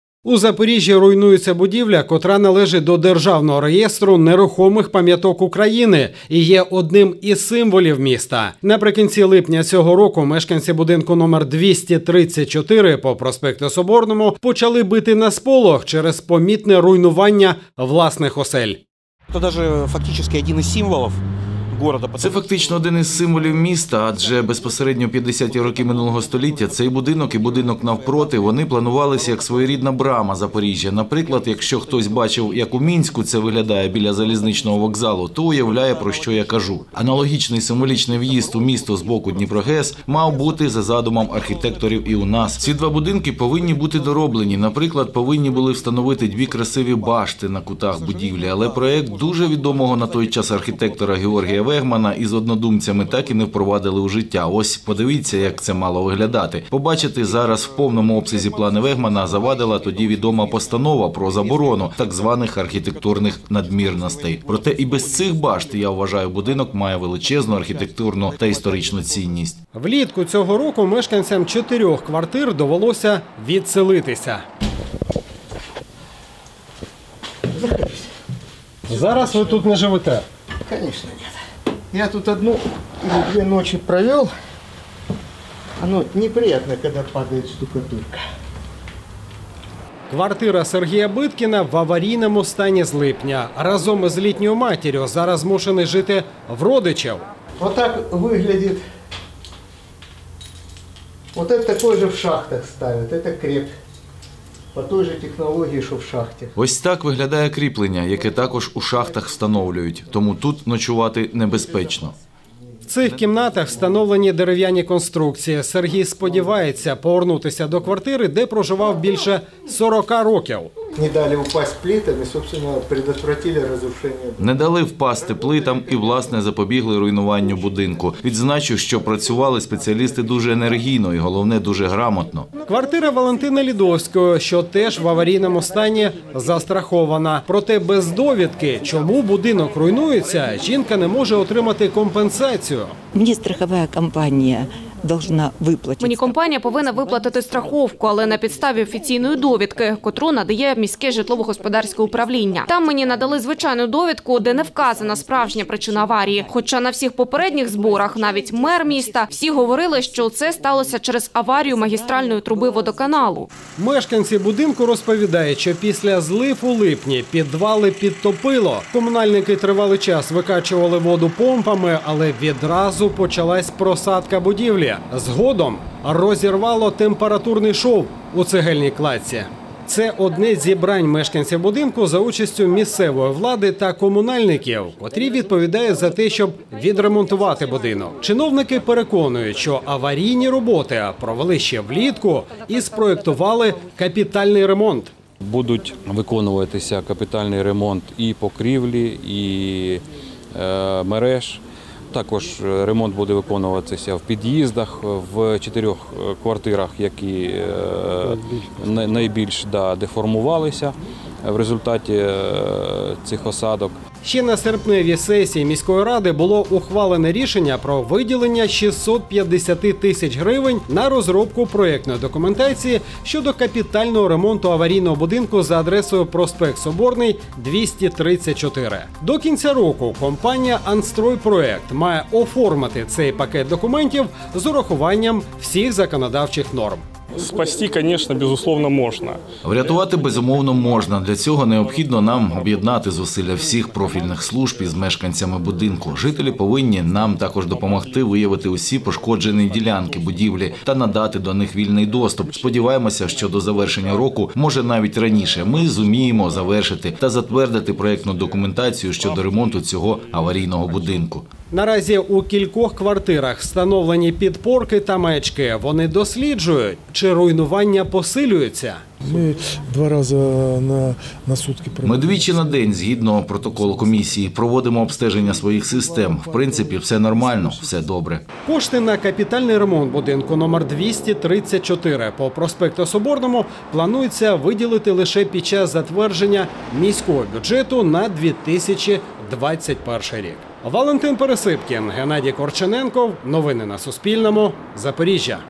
Новини - В Запоріжжі руйнується будинок (28.09.2020)
У Запоріжжі руйнується будівля, котра належить до державного реєстру нерухомих пам’яток України. Наприкінці липня цього року мешканці будинку номер 234 по проспекту Соборному почали бити на сполох через помітне руйнування власних осель. Жителям чотирьох найбільш деформованих квартир довелося відселитись. Як на часі вирішуються проблеми із порятунком будинку, котрий в архітектурному ансамблі Запоріжжя посідає особливе місце, з’ясовували кореспонденти Суспільного.